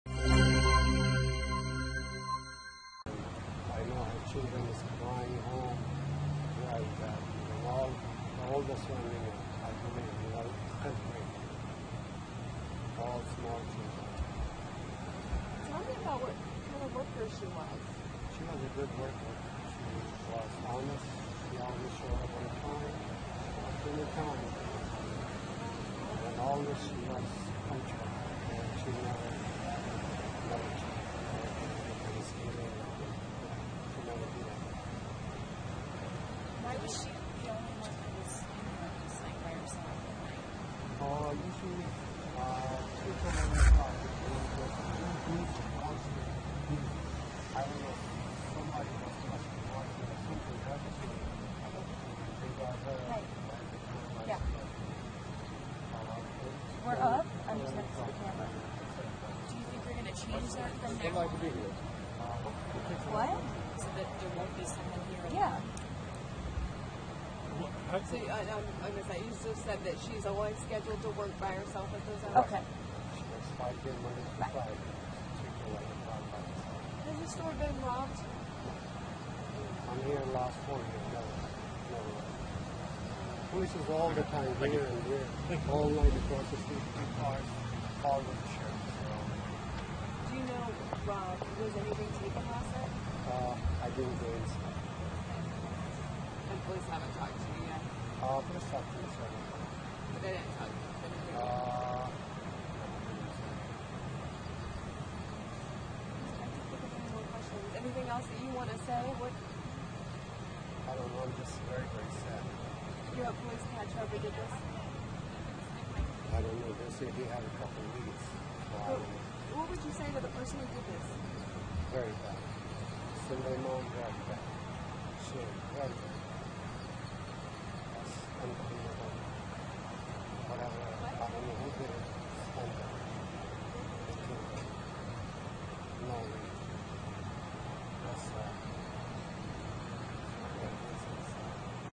Store owner saddened by death of employee
Razzi Enseri speaks about a store clerk who was gunned down inside his store in Palm Coast. Read the story